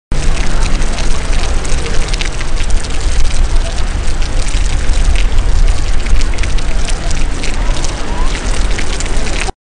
International Film & Video
Water pours from the mouths of statues in the Fountain of Neptune.